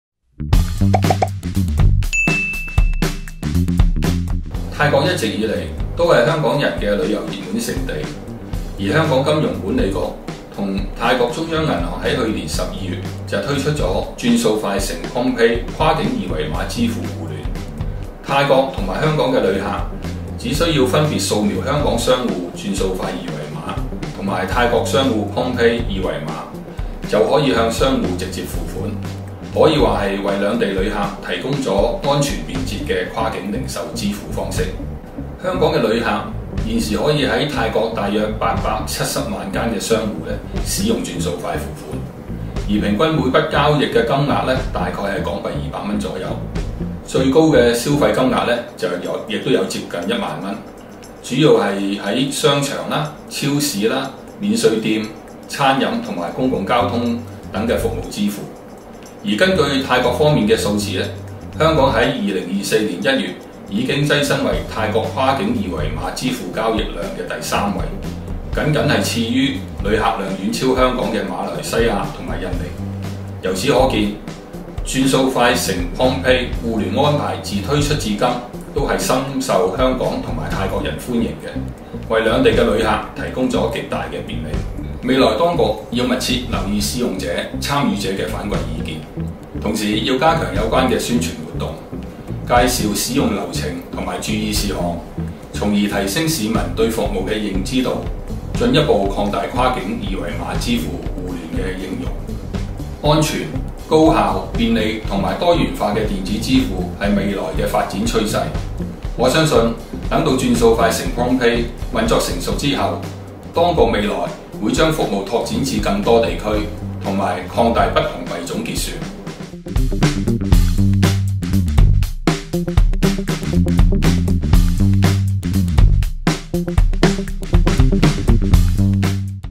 【3分鐘網紅】港人泰國遊可用「轉數快 x PromptPay」 陳沛良：安全 高效 便利 多元支付新趨勢
一直以來，泰國都是港人的熱門旅遊地點。立法會選委會界別議員陳沛良表示，去年12月香港金融管理局與泰國中央銀行推出「轉數快 x PromptPay」跨境二維碼支付互聯，泰國及香港旅客只要分別掃描香港商戶轉數快二維碼及泰國商戶PromptPay二維碼，就可向商戶付款，令消費時更加順暢及便利。